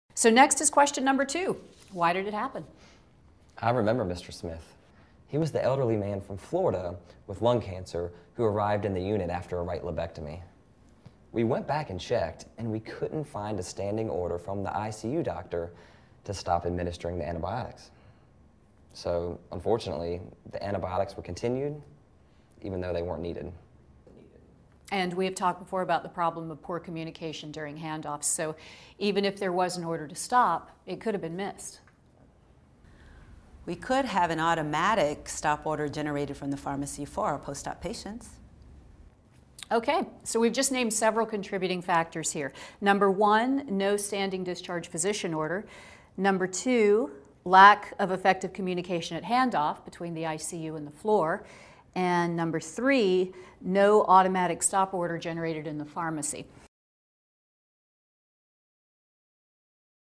CUSP: Identifying Contributing Factors
Identifying the factors that contributed to poor patient safety practices helps staff decide how to address them. See how this CUSP technique provides the clarity that’s needed to seek solutions.

The Comprehensive Unit-based Safety Program (CUSP) was developed by the Agency for Healthcare Research and Quality (AHRQ).